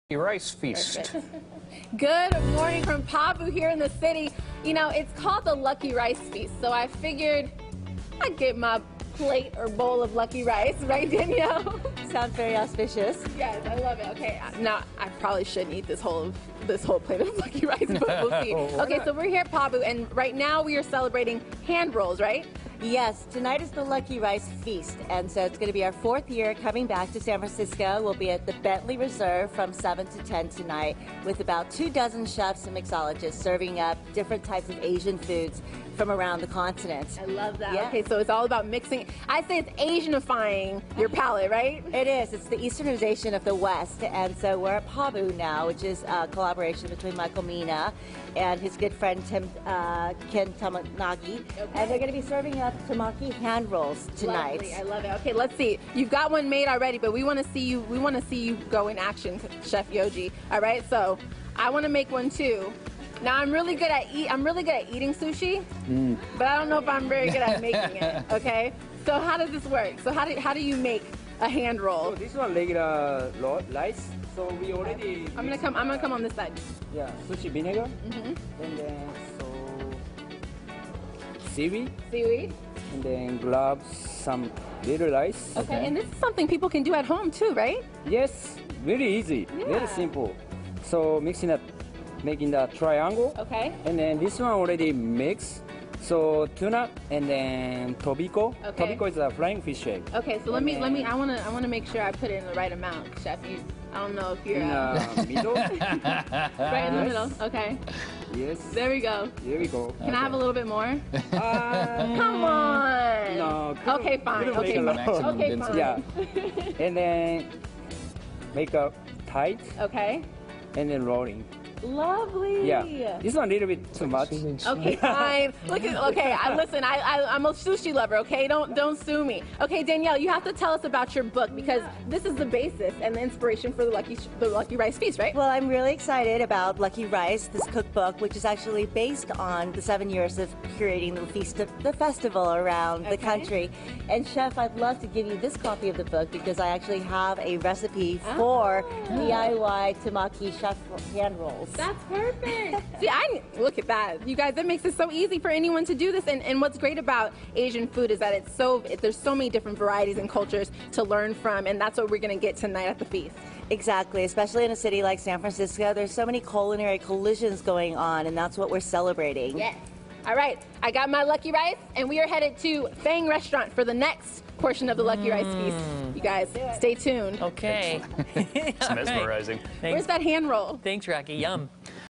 Luckyrice Feast
Roqui is in the bay this morning to tell us about a special feast that you're invited to tonight in the city!